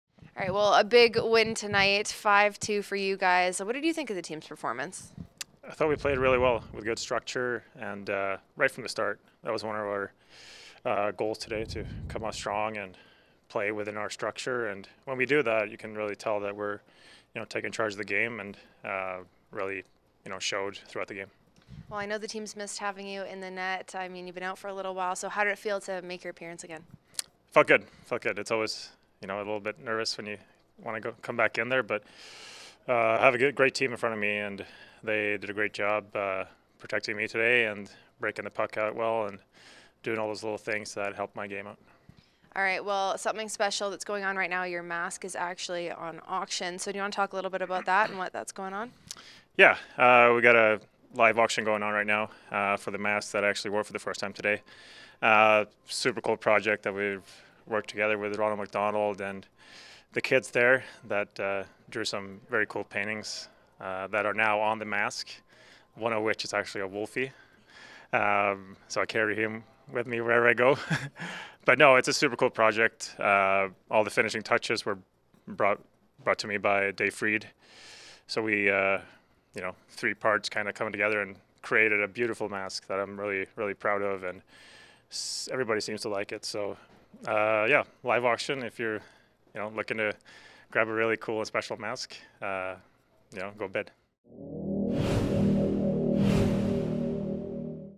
Postgame - Oscar Dansk - 03.07.2023
Oscar Dansk spoke to the media following a 5-2 win over the Eagles.